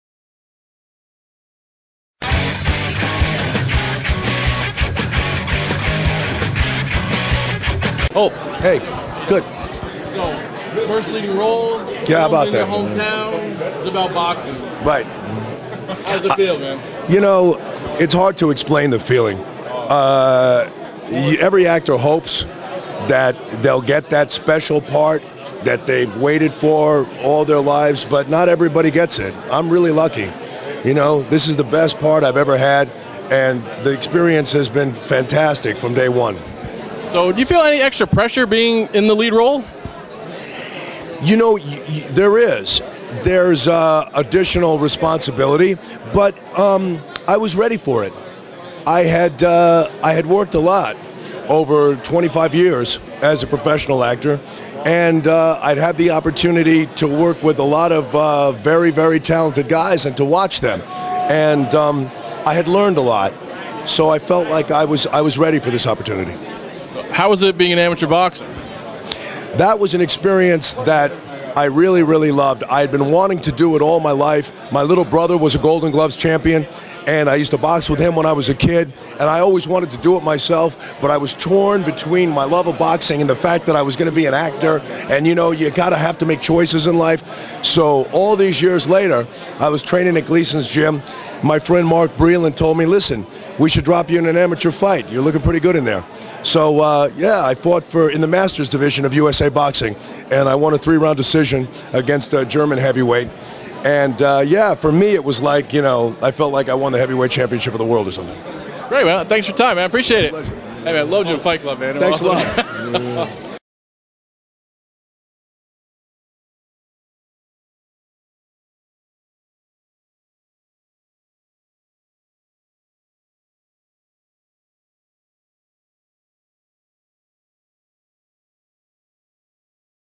Interview with actor Holt McCallany at the Lights Out Premiere
Ranu's Reviews was covering the red carpet at FX's Lights Out Premiere in NYC. I caught up with the star of the show, Holt McCallany who plays Patrick "Lights" Leary.

Holt has been in a lot of movies and TV shows, but my favorite role of his was in the movie Fight Club where he played the mechanic